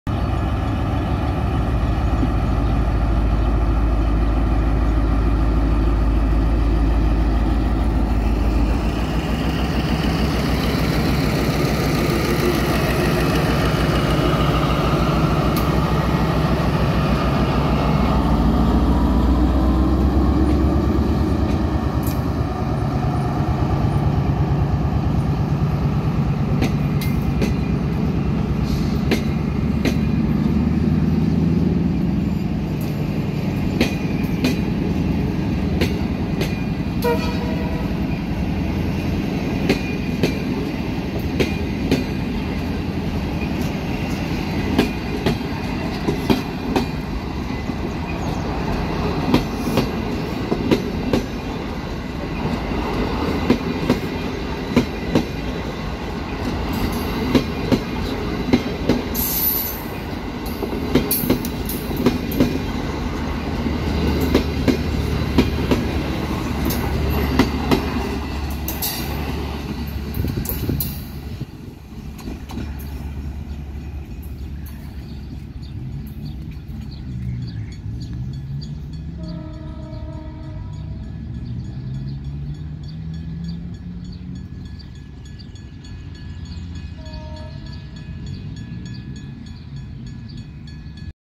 [4K] Tren de pasajeros de Trenes Argentinos Operaciones (SOFSE) saliendo hacia Córdoba
Luego de llegar y detenerse unos minutos para el ascenso y descenso de pasajeros Ahora la CNR Dalian Locomotive and Rolling Stock Co. Ltd. CKD 8H 0001 mas el tren de pasajeros procedente de Buenos Aires hacía su salida de aquí de la Ciudad de Marcos Juárez, continuando con su viaje hacia Córdoba Capital. Marcos Juárez. Córdoba. Argentina. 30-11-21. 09:50 a.m.